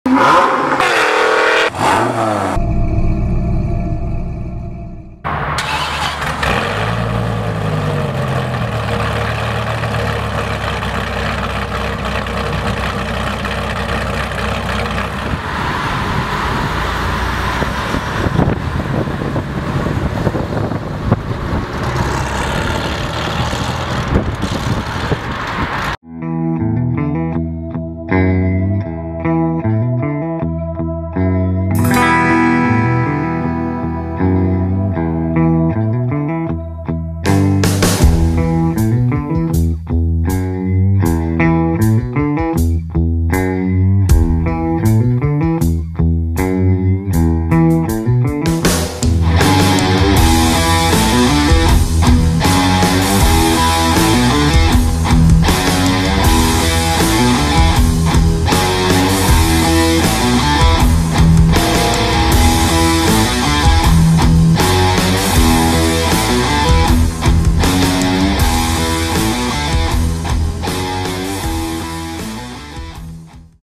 1999 Dodge Viper ACR (Modified)
**SOLD**

BJ Motors is proud to offer this 1999 Dodge Viper ACR in Viper Black with Silver Stripes over a Custom Black and Cream Interior. This ACR comes to us with 35,005 miles from new and is in amazing condition. This ACR is loaded with custom upgrades that make it stand out from the crowd, including a fully custom Black and Cream Alcantara and Leather Interior as well as a custom audio system. See below for a full is of upgrades on this beast!

Upgrades:

Penske Coilovers
Aftermarket Camshaft
Asanti Wheels
Michelin PS4S Tires
Header-back Exhaust
Custom Suede Interior
Custom Audio System